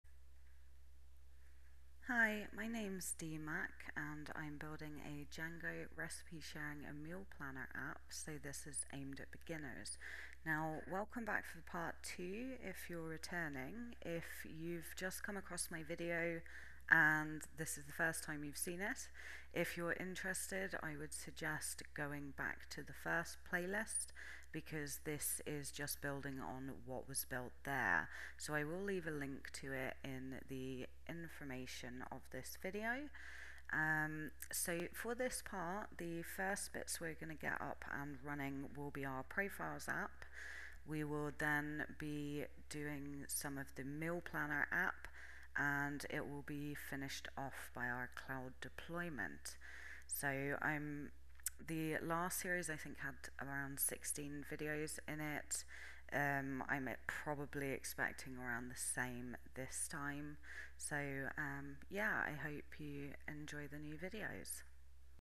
Django Recipe Sharing Tutorial - Part 2
Part 2 of the django Recipe Sharing & Meal Planner App

If you've just found this video and want to learn more and code along, you should start with part 1 of the series